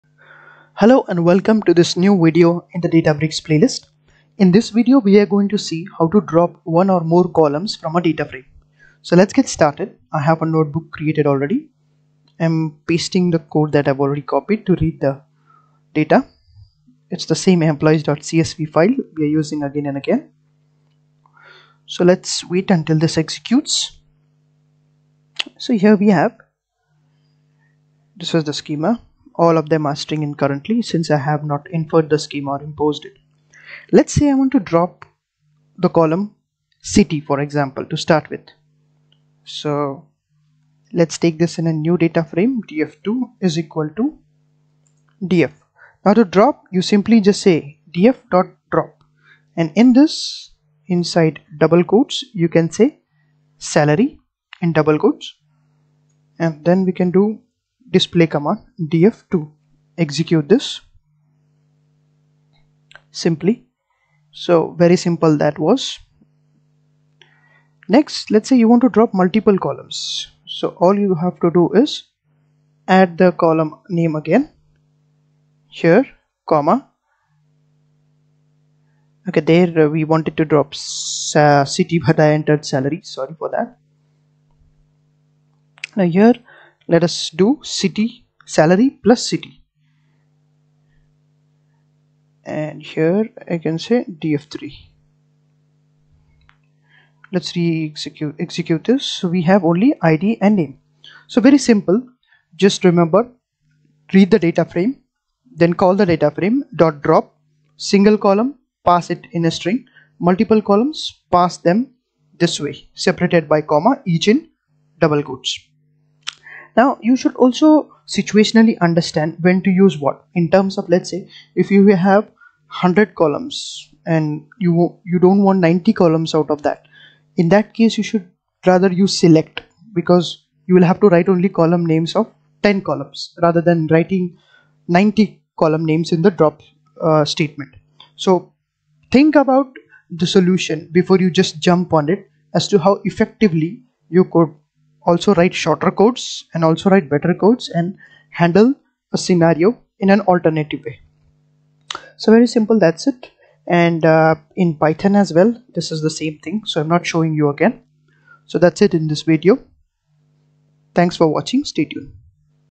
14. Drop Columns From DataFrame
Hello everyone,
This video covers how to drop columns from a dataframe.

For this example use [Employees.csv] file in [008] folder present in Google drive:

Other playlists in channel:-